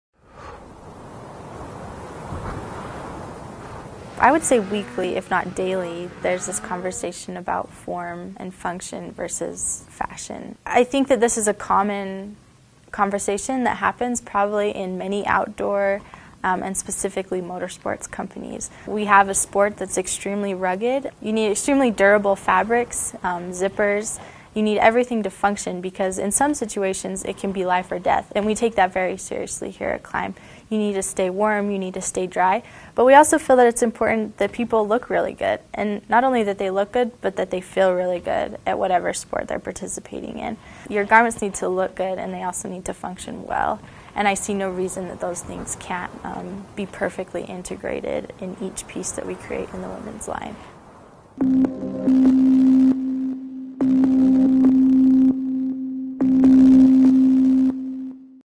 Function vs. Fashion KLIM Womens Gear
Take a look at what the professionals at KLIM have to say about their engineered womens gear for 2013.  They have gone above and beyond with there products in the Womens line this year combining both Function and Fashion.